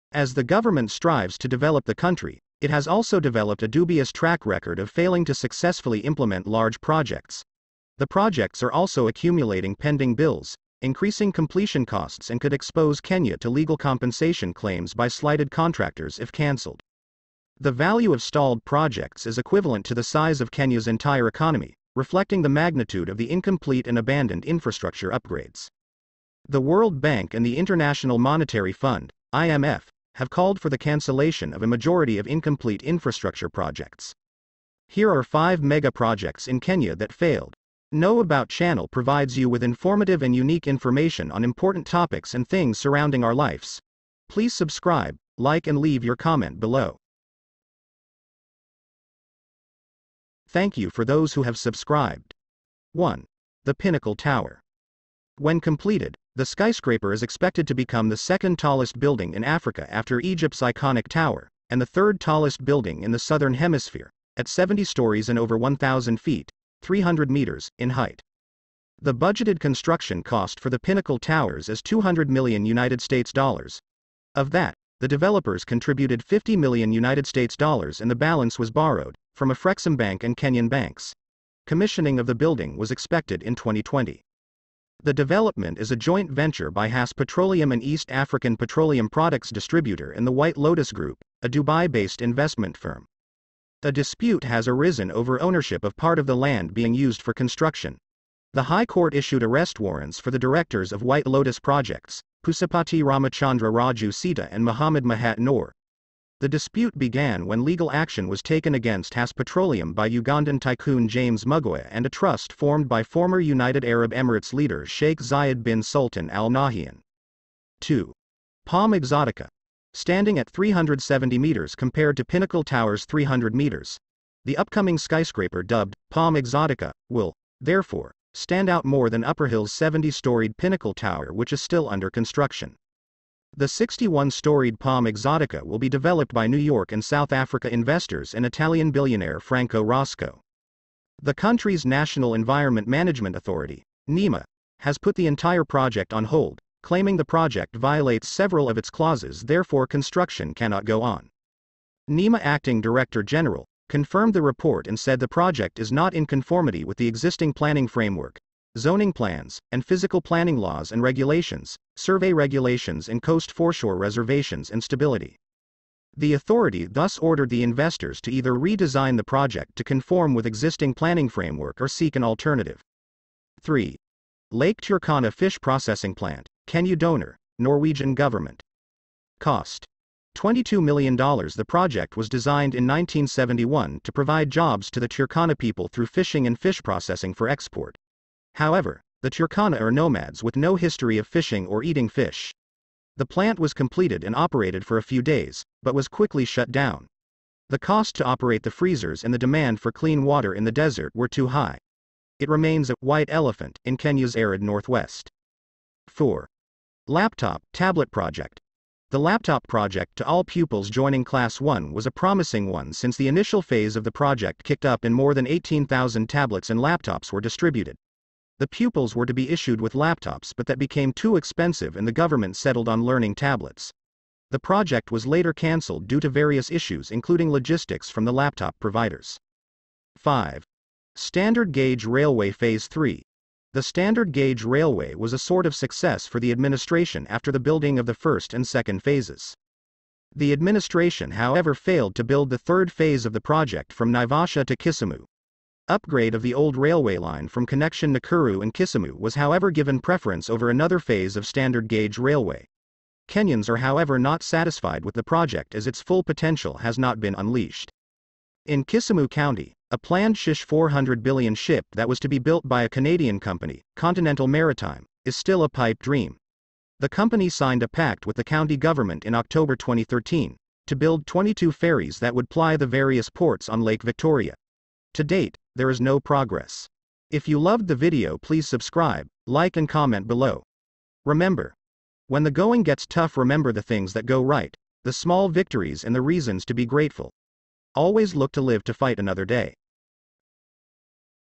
multi-billion projects that never took shape in kenya 🇰🇪
The Pinnacle tower.

When completed, the skyscraper is expected to become the second tallest building in Africa after Egypt's Iconic Tower, and the third tallest building in the Southern Hemisphere, at 70 stories and over 1,000 feet (300 m) in height.

The budgeted construction cost for the Pinnacle Towers is US$200 million. Of that, the developers contributed US$50 million and the balance was borrowed, from Afreximbank and some Kenyan banks. The main contractor is China State Construction Engineering Corporation (CSCEC). Commissioning of the building was expected in 2020.

The development consists of two adjacent towers. The shorter tower, at 46 floors will house the 257-room, five-star Upper Hill Hilton Hotel, the third Hilton franchise in Nairobi and the 50th on the African continent.[3] The taller tower, with 70 floors, will feature eleven floors of commercial office space, nine floors of upscale rental retail space, and 46 floors with 210 upscale 1, 2 and 3-bedroom, serviced residential apartments.

The development is a joint venture by Hass Petroleum an East African petroleum products distributor and the White Lotus Group, a Dubai-based investment firm.

A dispute has arisen over ownership of part of the land being used for construction.[11] The High Court has issued arrest warrants for the directors of White Lotus Projects, Poosapati Ramachandra Raju Sita and Mohamud Mahat Noor.[11] The dispute began when legal action was taken against Hass Petroleum by Ugandan tycoon James Mugoya and a trust formed by former United Arab Emirates leader Sheikh Zayed Bin Sultan Al Nahyan.

2. Palm Exotica.

standing at 370 metres compared to Pinnacle Tower’s 300 metres. The upcoming skyscraper dubbed ‘Palm Exotica’ will, therefore, stand out more than Upperhill’s 70-storeyed Pinnacle Tower which is still under construction.

The 61-storeyed Palm Exotica will be developed by New York and South Africa investors and Italian billionaire Franco Rosco.

The country’s National Environment Management Authority (Nema) has put the entire project on hold, claiming the project violates several of its clauses therefore construction cannot go on.

Nema Acting Director General, Mamo Mamo confirmed the report and said the project is not in conformity with the existing planning framework: Zoning plans, and physical planning laws and regulations, survey regulations and Coast foreshore reservations and stability.

The authority thus ordered the investors to either re-design the project to conform with existing planning framework or seek an alternative site.

Where it went wrong: The project was designed in 1971 to provide jobs to the Turkana people through fishing and fish processing for export. However, the Turkana are nomads with no history of fishing or eating fish. The plant was completed and operated for a few days, but was quickly shut down. The cost to operate the freezers and the demand for clean water in the desert were too high. It remains a “white elephant” in Kenya’s arid northwest.

Laptop/Tablet project.

The Laptop project to all pupils joining class 1 was a promising one since the initial phase of the project kicked up and more than 18000 tablets and laptops were distributed. The pupils were to be issued with laptops but that became too expensive and the government settled on learning tablets. The project was later canceled due to various issues including logistics from the laptop providers.

Dams Projects.

The dam projects was another very ambitious mega project under Jubilee administration as they came to power. The project was to see construction of Dams across the country including Itare Dam in Nakuru, Arror and Kimwarer Dams in Elgeyo Marakwet county and Thiba Dam in Central Kenya just to name a few. Only the Thiba dam was substantially build but others like Arror, Kimwarer and Itare all failed.

Standard Gauge railway Phase 3.

The standard G auge Railway was a sort of success for the administration after the building of the first and second phases. The administration however failed to build the third phase of the project from Naivasha to Kisumu. Upgrade of the old railway line from connection Nakuru and Kisumu was however given preference over another phase of Standard Gauge railway. Kenyans are however not satisfied with the project as its full potential has not been unleashed.